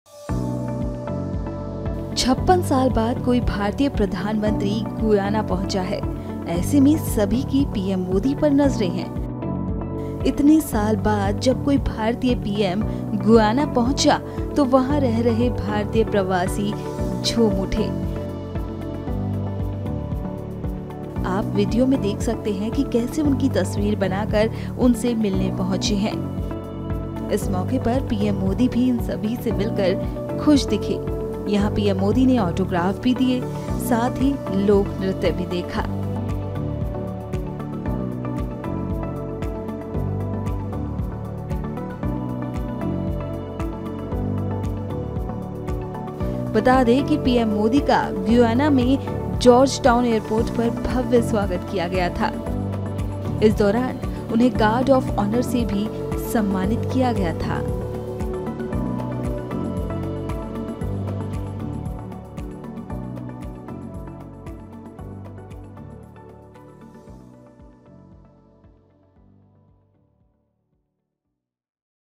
Guyana पहुंचे PM Modi, झूम उठे भारतीय, चित्र ने खींचा सबका ध्यान
56 साल बाद कोई भारतीय प्रधानमंत्री गुयाना (Guyana) पहुंचा हैं...ऐसे में सभी की पीएम मोदी (PM Modi) पर नजरें हैं...इतने साल बाद जब कोई भारतीय पीएम गुयाना पहुंचा तो वहां रह रहे प्रवासी भारतीय झूम उठे...